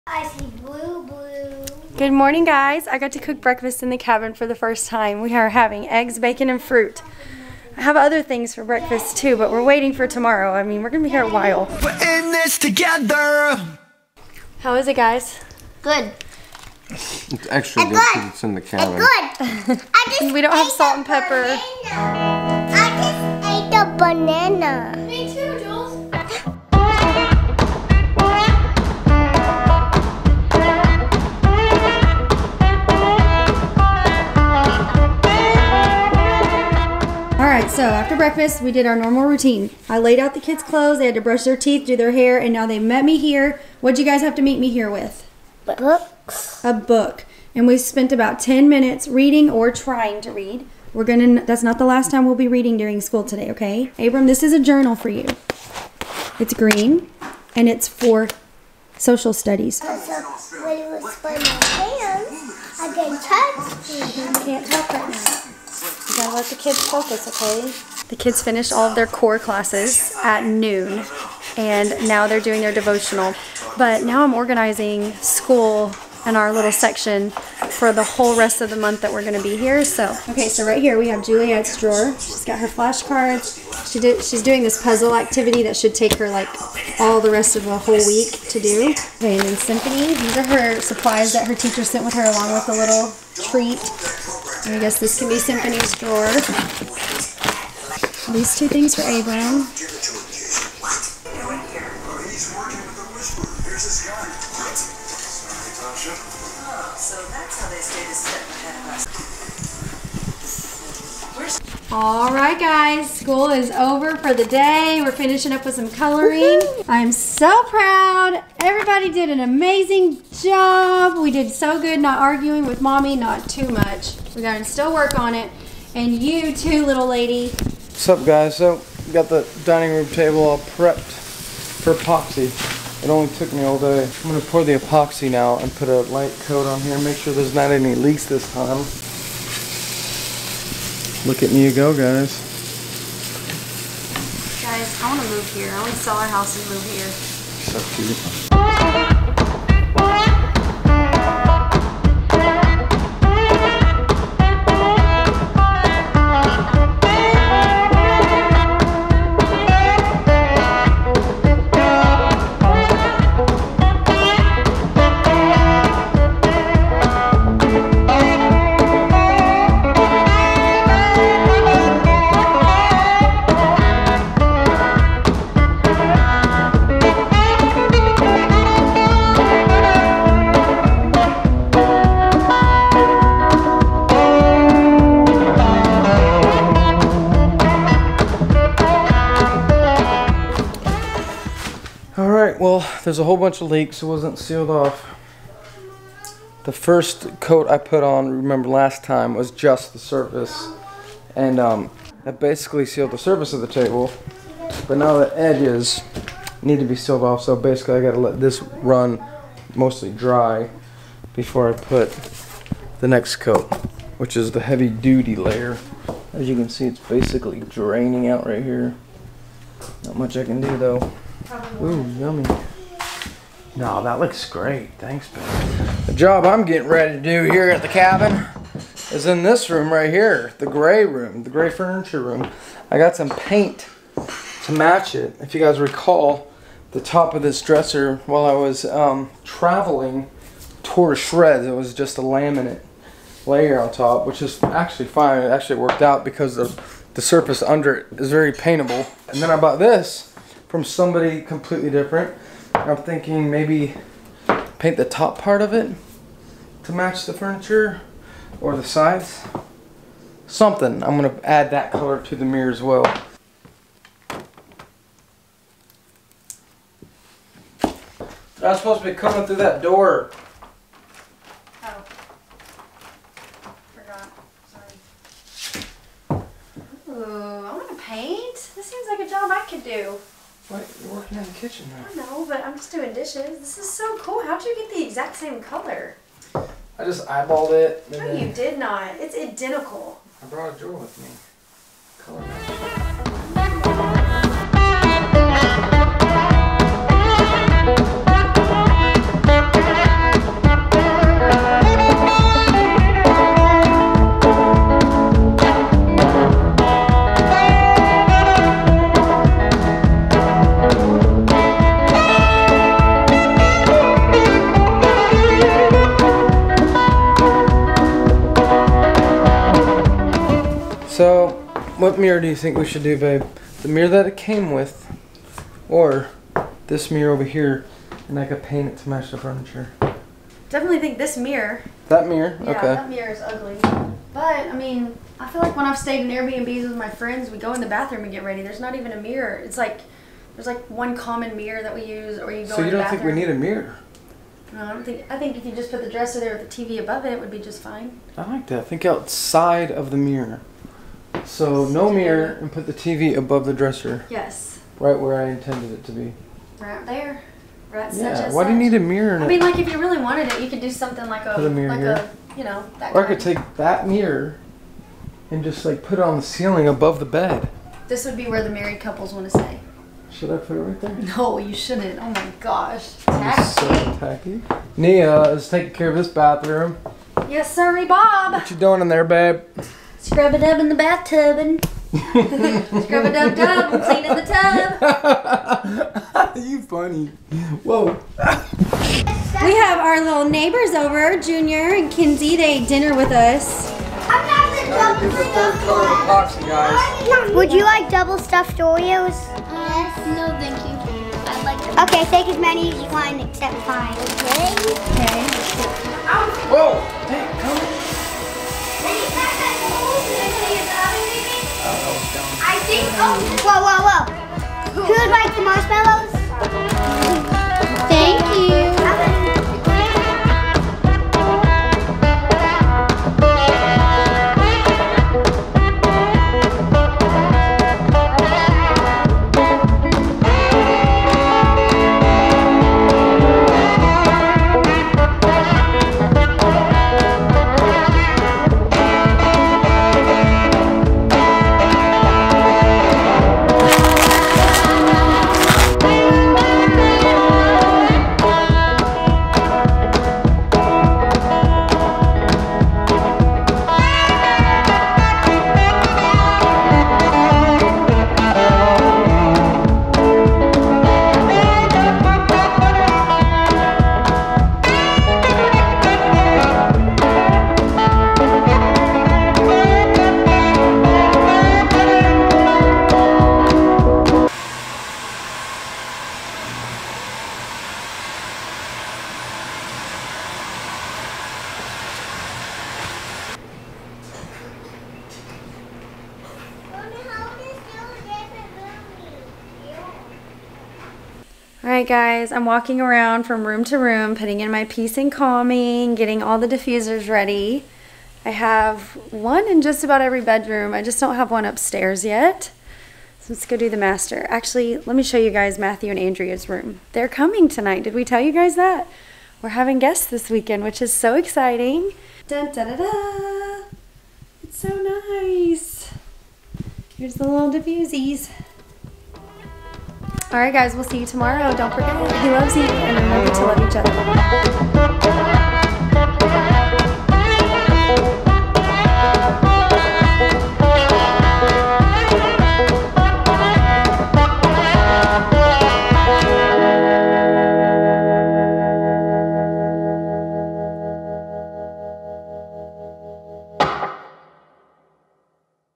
First Day of Home School!
We're settled in the cabin and it's now time to start teaching! Today is the our first day of home school and we are all loving it!